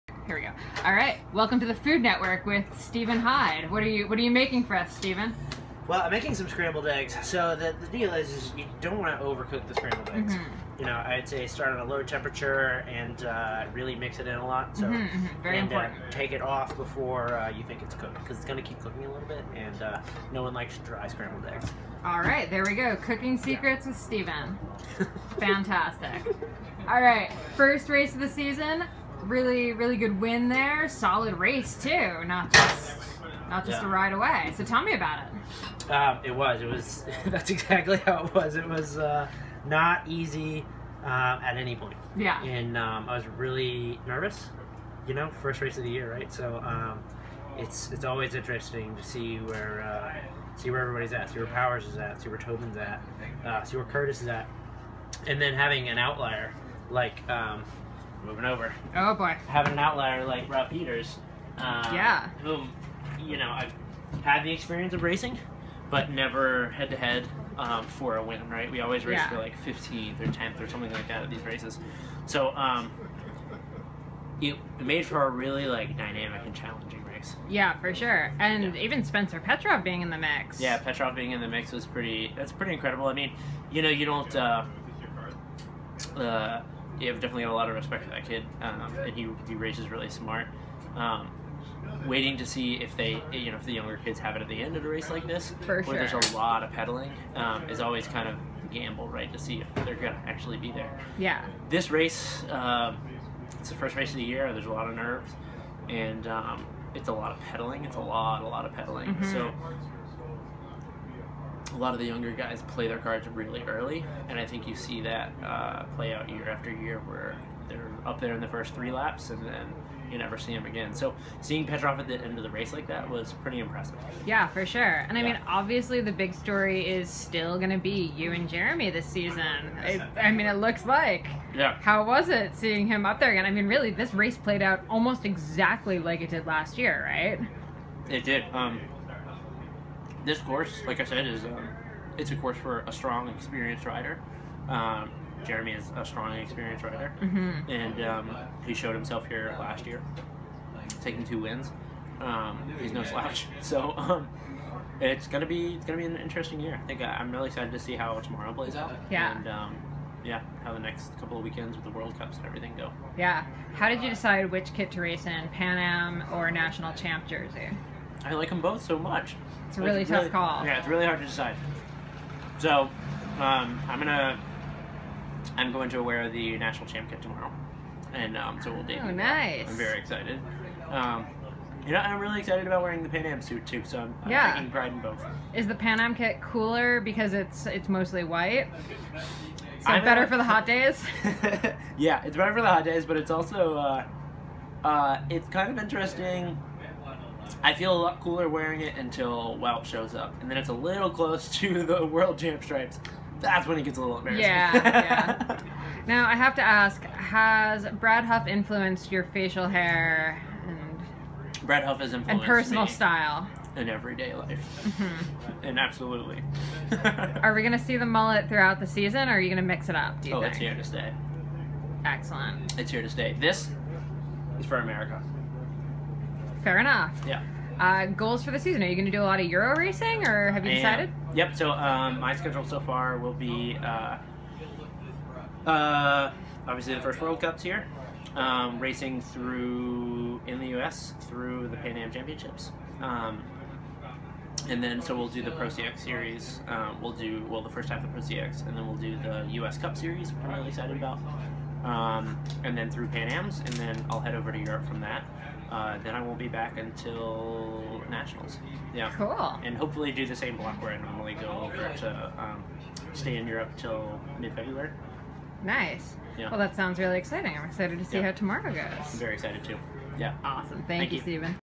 Stephen Hyde Wins At Rochester Cyclocross
Stephen Hyde scrambles some eggs and breaks down his race at Rochester Cyclocross along with his hair inspiration and season schedule.